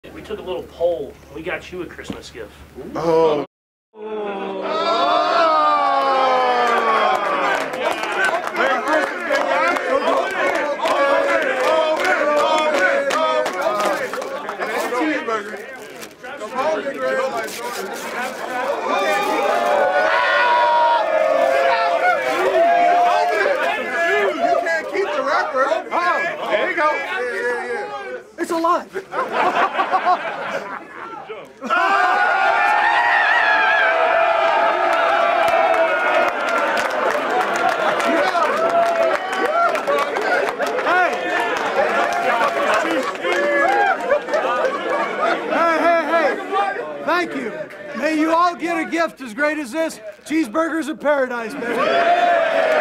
PRESENT REVEAL‼️ What's in the box, Coach? 🎁 | Kansas City Chiefs
The guys took a poll and the gift to Coach Reid was pretty unanimous...